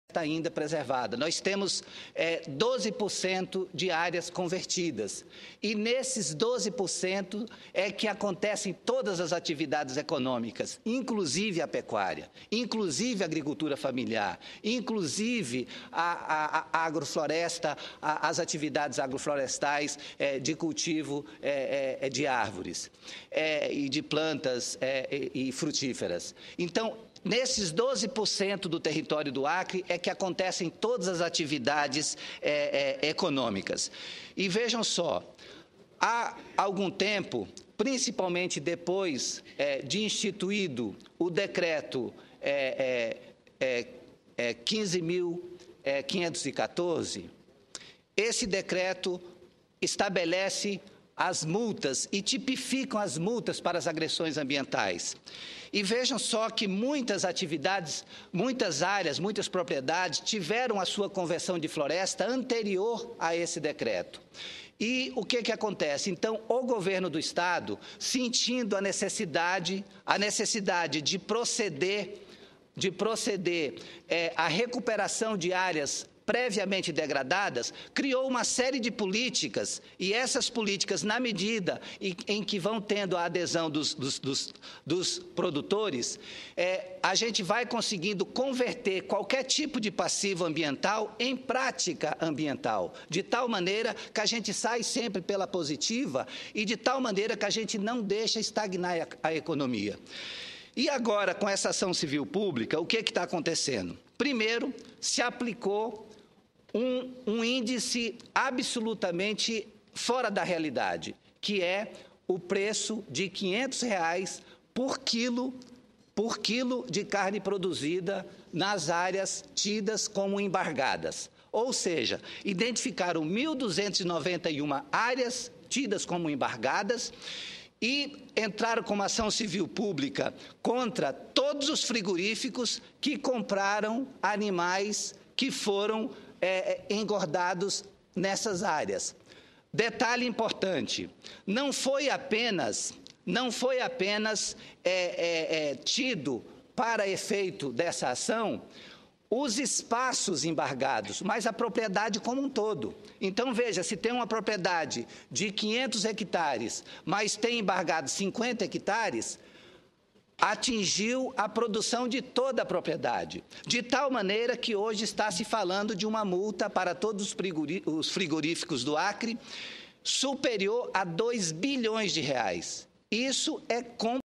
Sen. Aníbal Diniz adverte que ação civil pública deve impor multa a todos os frigoríficos do Acre
25/04/2011 - Plenário - Sen. Aníbal Diniz adverte que ação civil pública deve impor multa a todos os frigoríficos do Acre